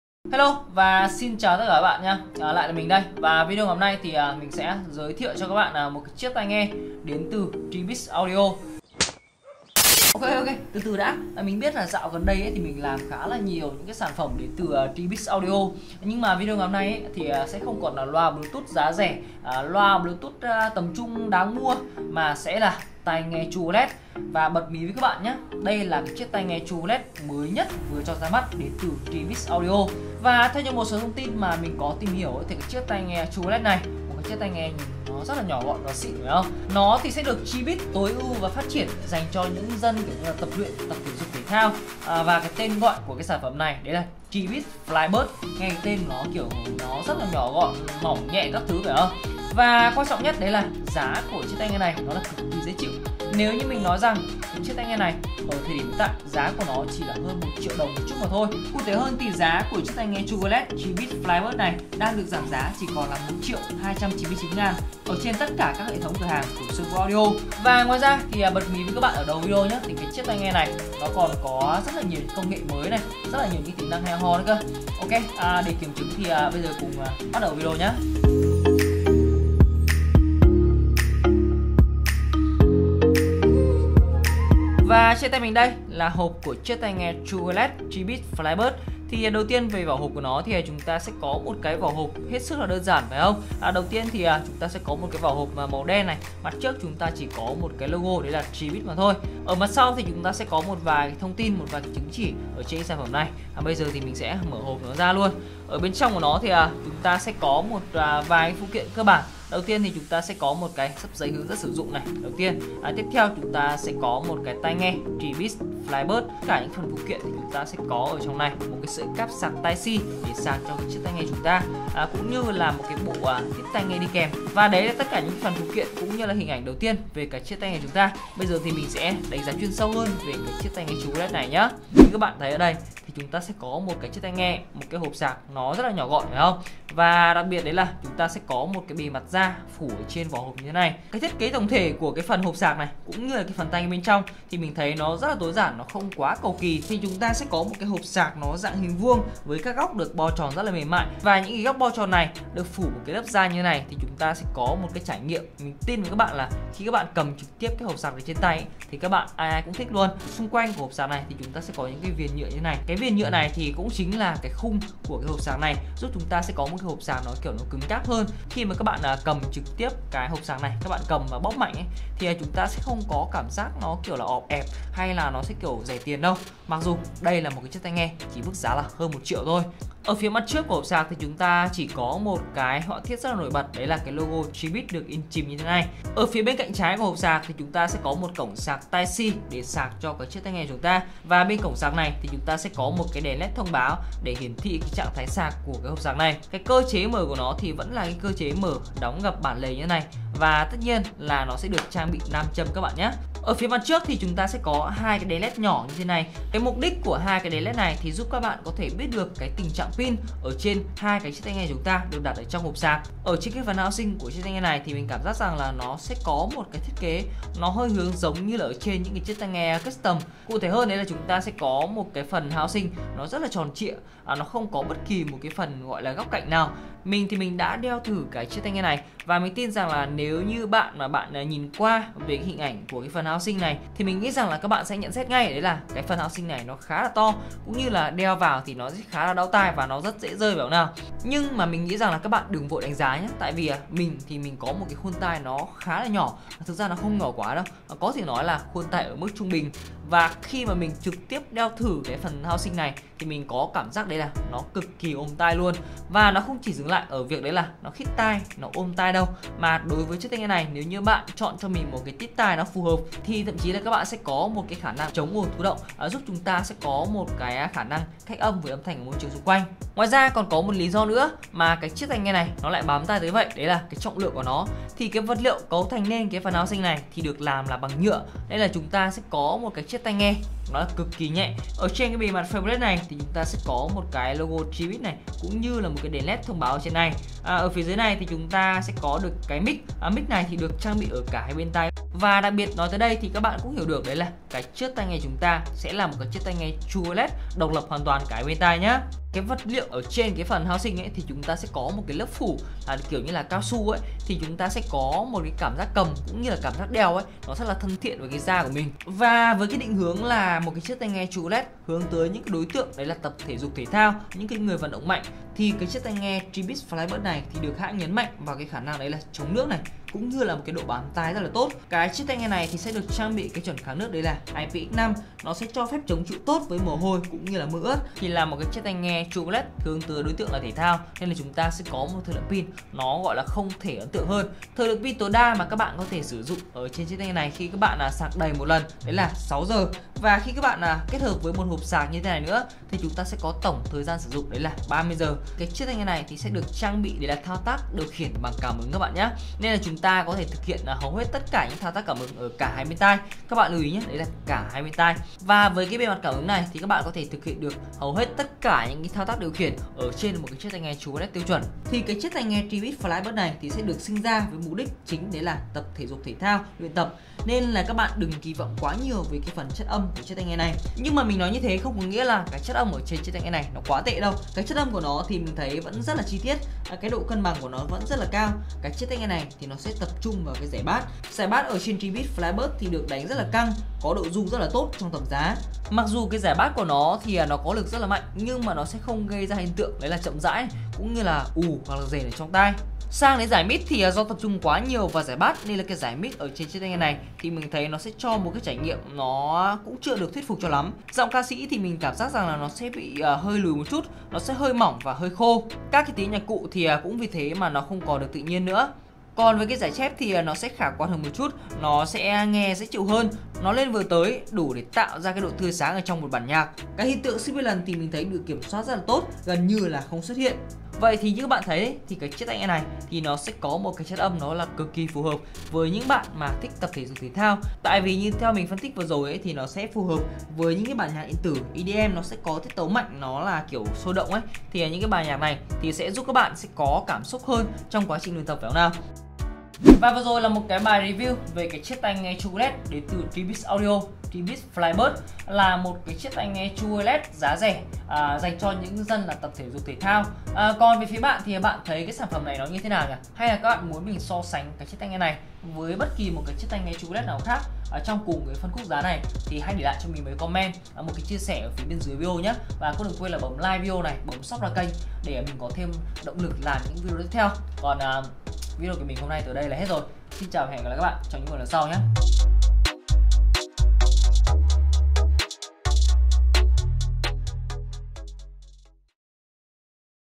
Trên tay tai nghe True Wireless Tribit FlyBuds | Tai nghe cho người YÊU THỂ THAO
👇Truy cập thêm thông tin  👇
🌐Website Xuan Vu Audio ©️
👨‍👨‍👦‍👦 Tham gia cộng đồng người yêu tai nghe để biết thêm nhiều thông tin